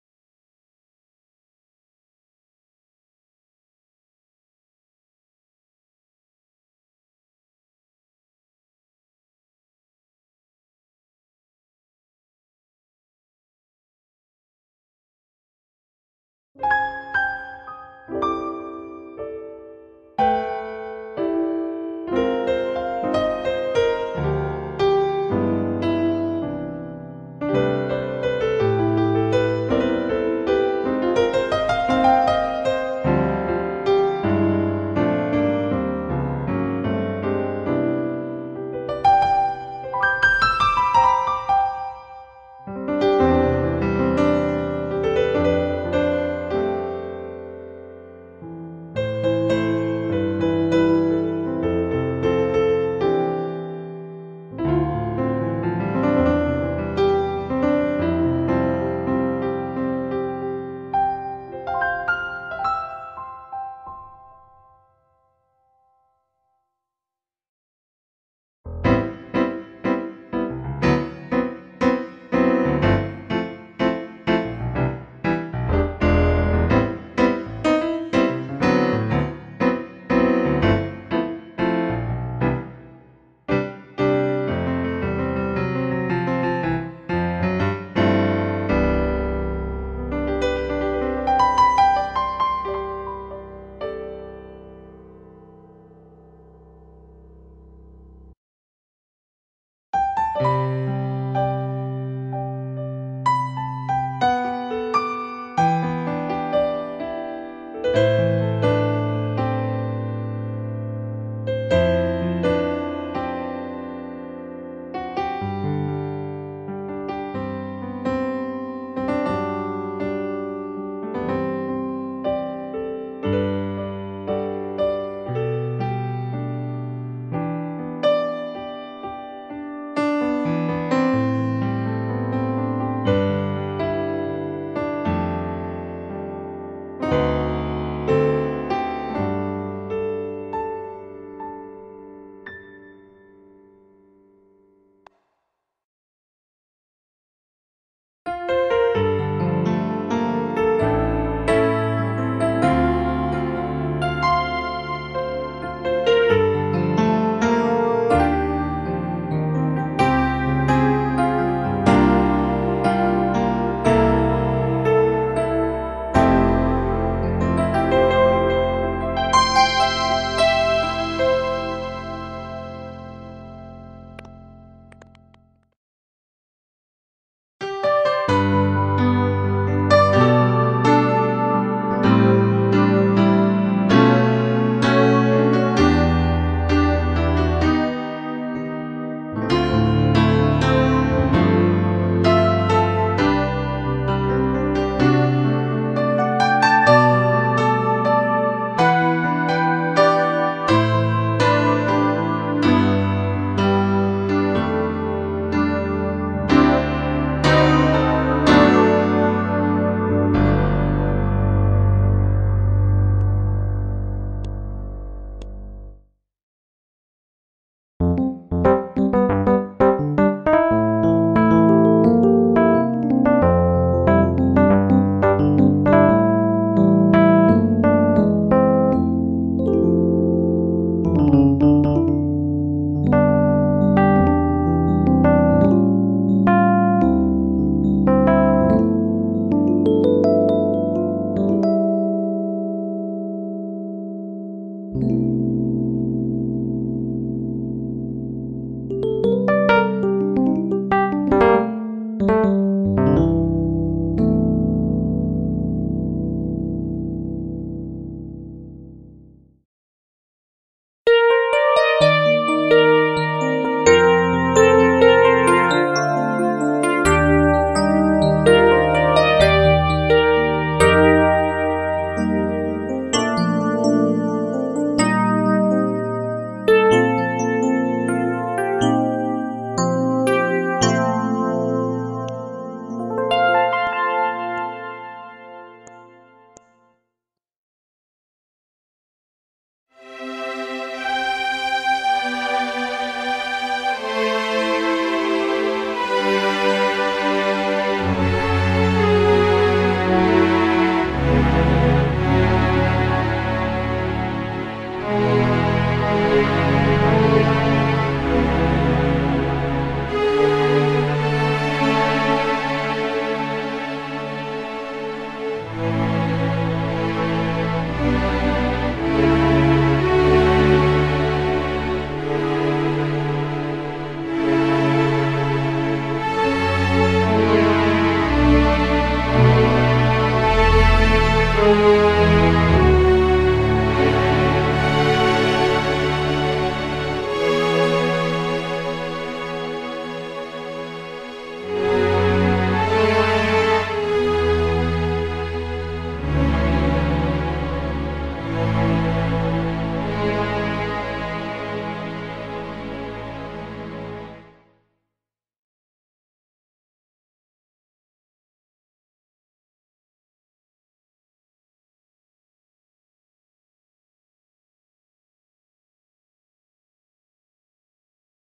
Pack Montage para Audio Evolution | (Pianos, Strings, Pads, Sanfonas, Orgãos e muito mais!)
Estou disponibilizando esse SUPER PACK sampleado por mim,
Diretamente de um Montage!

Timbres com Qualidades Absurdas com muitooo peso no PA. :D

O Pack Montage contem 14 Timbres.

Deus Abençoe todos!!